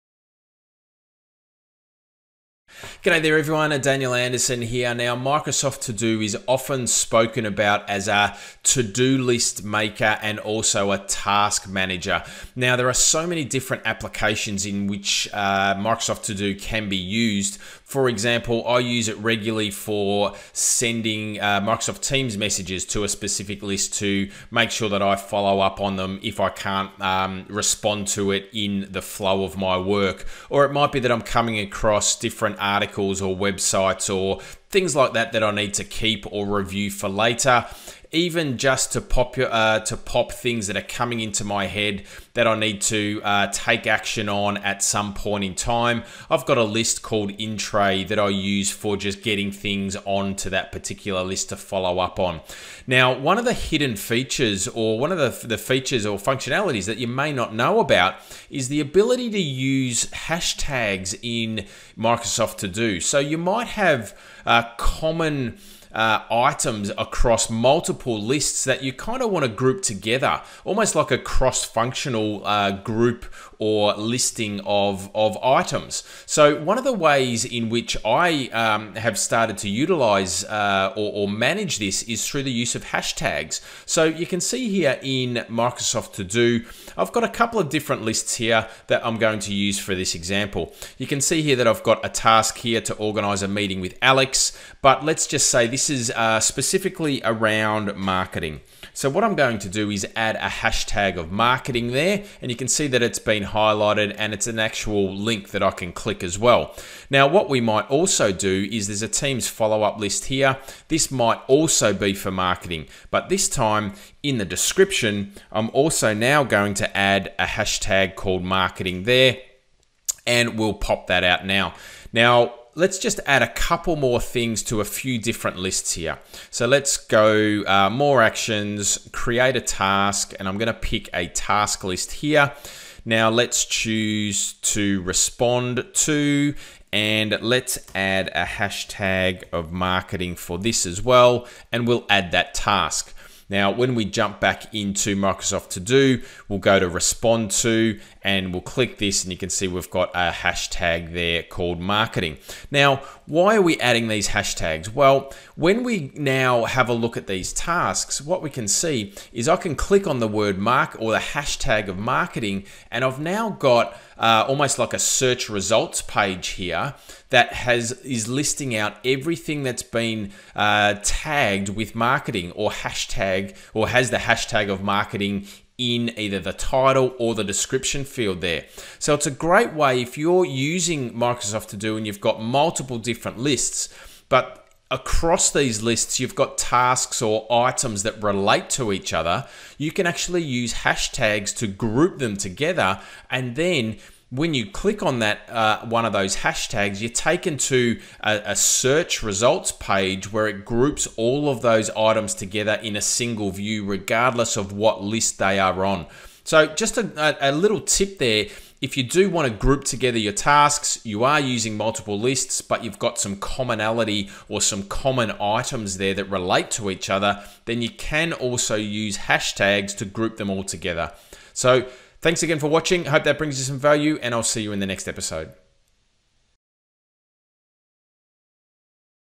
How to use hash tags in Microsoft To Do to your advantage
Microsoft To Do, as you may know is looked at as a daily planner, to do list and or task list. I think and know that it can be used for much more than that. One cool thing is that you can use hash tags. did you know that ?

Really handy for searching 😜...